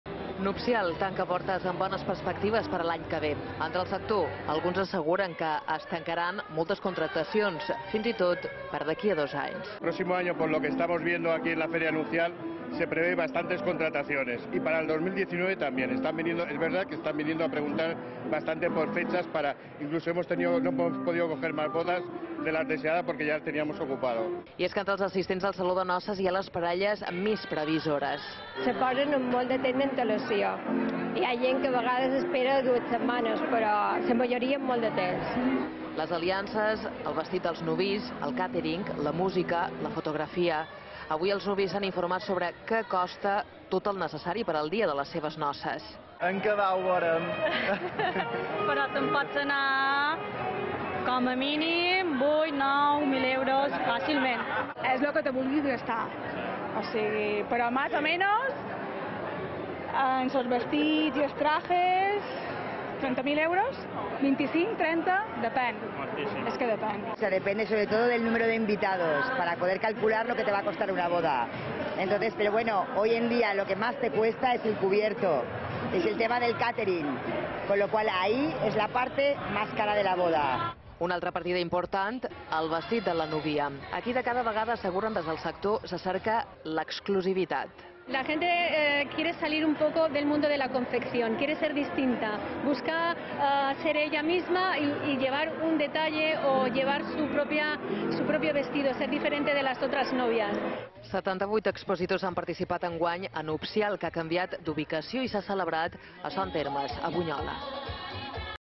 Nupcial tanca portes amb bones perspectives per a l’any que ve
La fira Nupcial tanca portes avui vespre després de 3 dies oferint als nuvis l’oferta existent en l’organització de noces a les Illes. Enguany el saló ha canviat d’escenari i s’ha traslladat a Son Termes, a Bunyola.